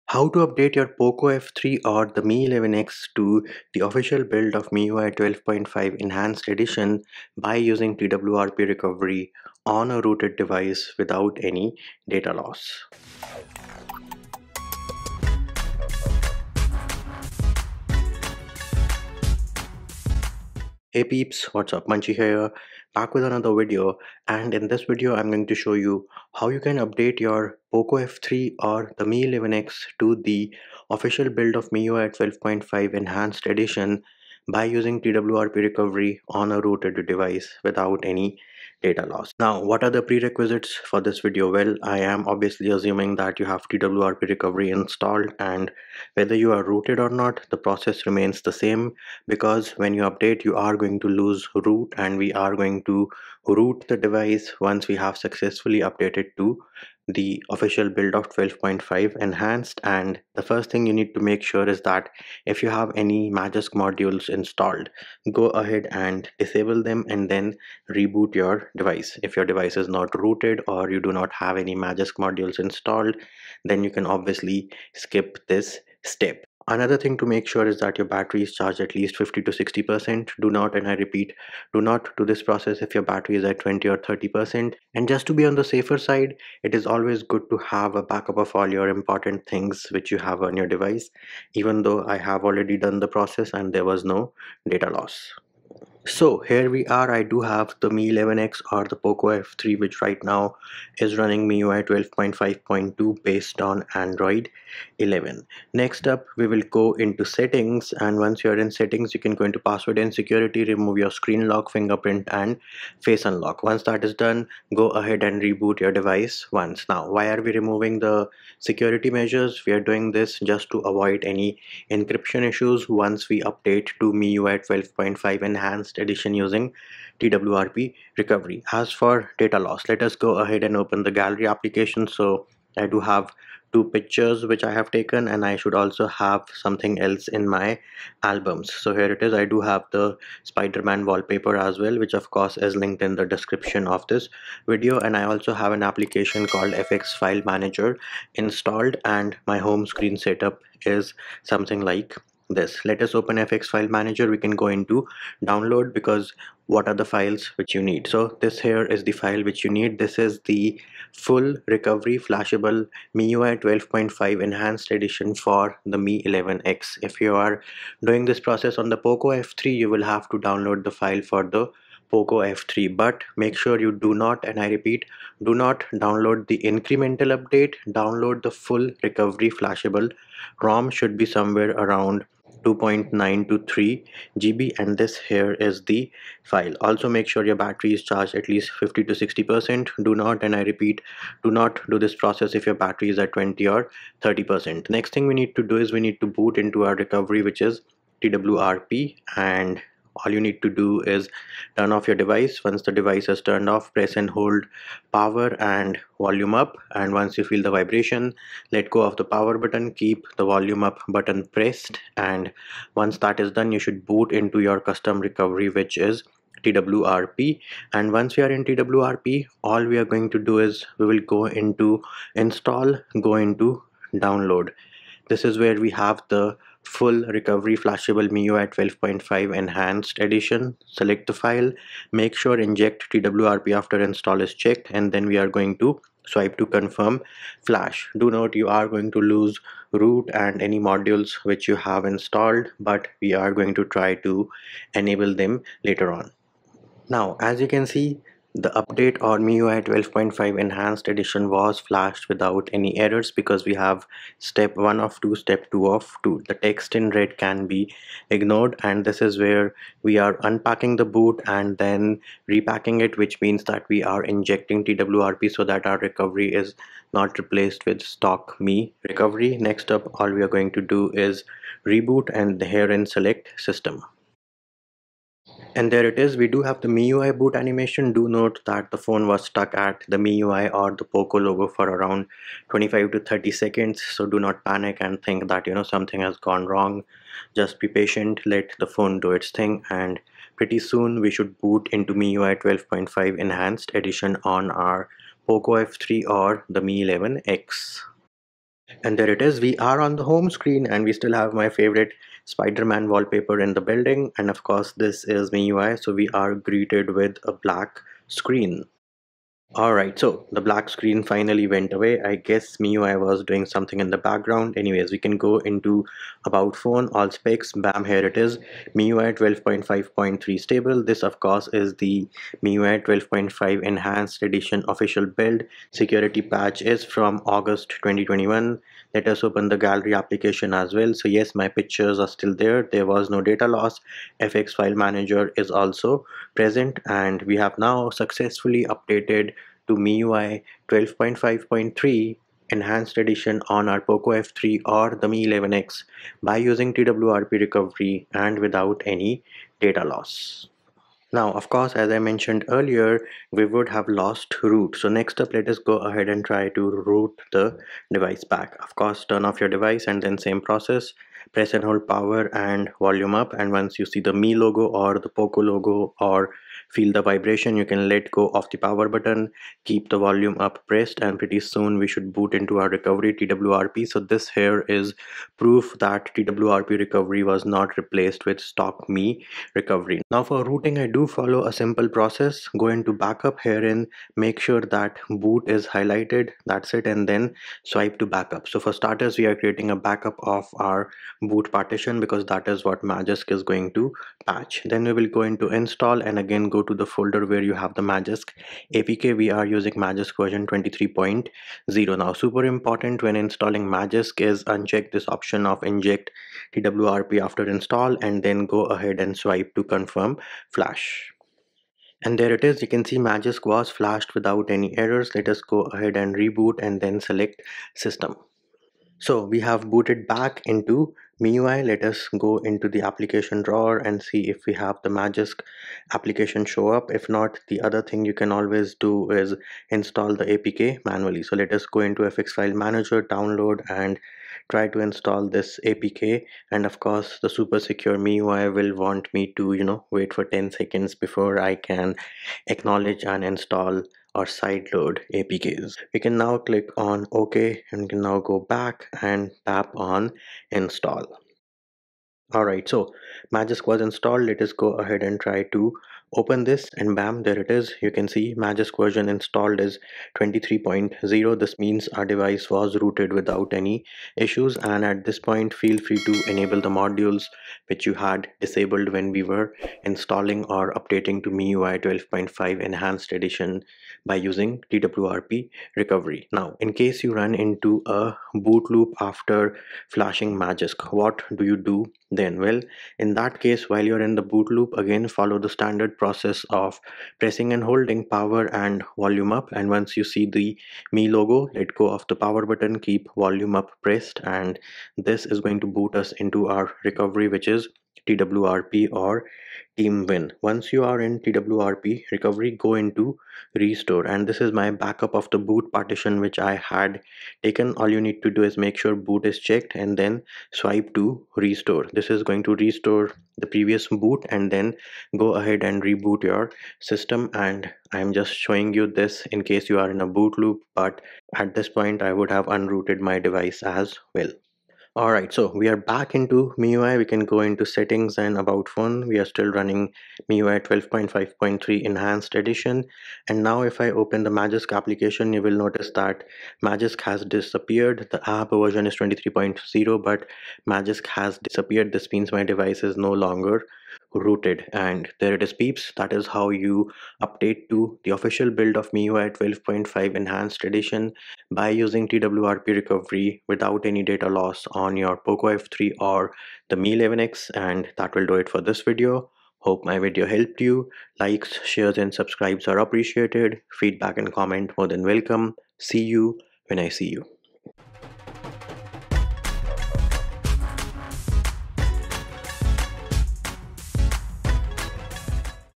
Xiaomi Mi 11X | Install Official Miui 12.5.3.0 Enhanced Edition | TWRP | NO Data Loss | Poco F3
Do you want to learn how to install the official stable Miui 12.5 Enhanced Edition (Miui 12.5.3.0) on the Xiaomi Mi 11X and Poco F3 without data loss and with a custom recovery like TWRP. In this detailed video tutorial we will show you how to install official stable Miui 12.5 Enhanced Edition (Miui 12.5.3.0) on the Xiaomi Mi 11X and Poco F3 without data loss and with a custom recovery like TWRP.

Poco F3 & Xiaomi Mi 11X Official Miui 12.5 Enhanced Edition Download Links

**Xiaomi Mi 11X Download Official Miui 12.5.3 Enhanced Edition**
Xiaomi Mi 11X Official Stable Miui 12.5.3 Enhanced Edition (Full Recovery ROM)
Filesize :- 2.9G
md5 :- 1f144638a5198fcc3f2ed72a93e6a4ea

**Poco F3 Global Download Official Miui 12.5.3 Enhanced Edition**
Poco F3 Official Stable Miui 12.5.3 Enhanced Edition (Full Recovery ROM)
Filesize :- 2.9G
md5 :- b628b9f893b7c5abc6426b4795eaf6e1